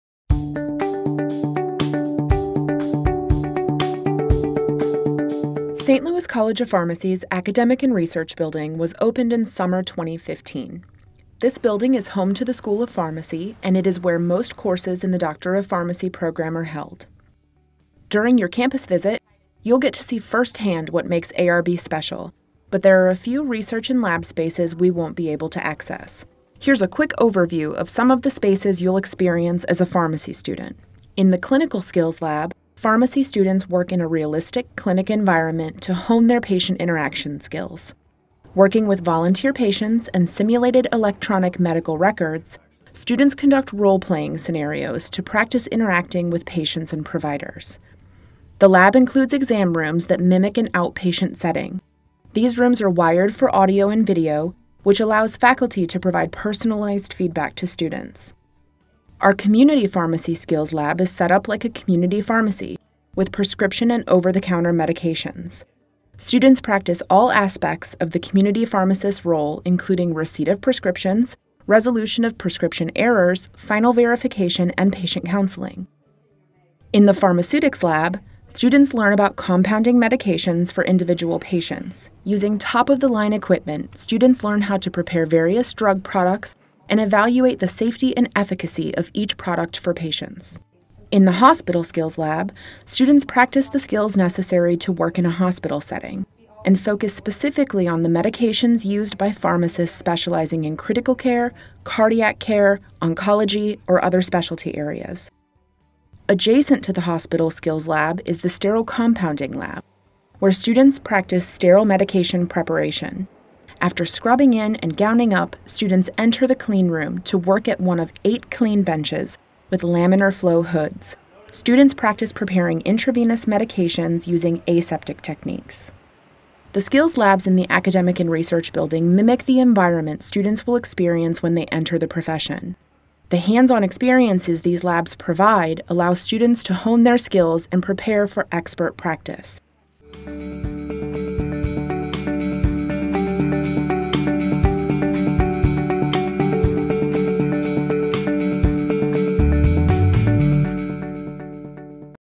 Sneak Peek: Pharmacy Skills Labs
When you visit campus, there are a few laboratory spaces we won't be able to show you. In this video, we take a behind-the-scenes look into the labs you'll work in as a pharmacy student.